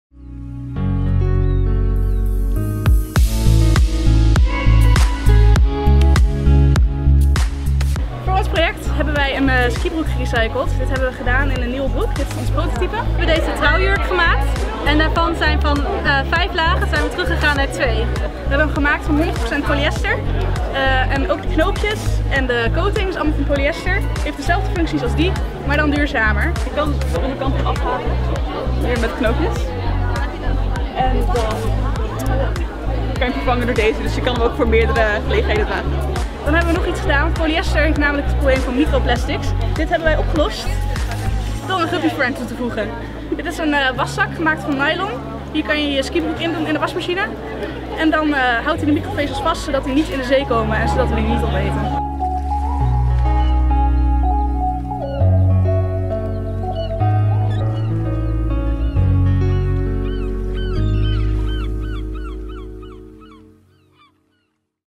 Fashion-studenten presenteren eindresultaat Materials II-project
Duurzaamheid is een urgente kwestie, vooral in de textiel- en kledingindustrie waar consumptie en afvalstromen blijven stijgen. 👗 In het Materials II-project ligt de focus erop dat tweedejaars studenten van de bacheloropleiding Fashion and Textile Technologies van Saxion leren om de impact van een bestaand kledingstuk of accessoire systematisch te evalueren om de ecologische voetafdruk te verbeteren. Deze week presenteerden ze het eindresultaat.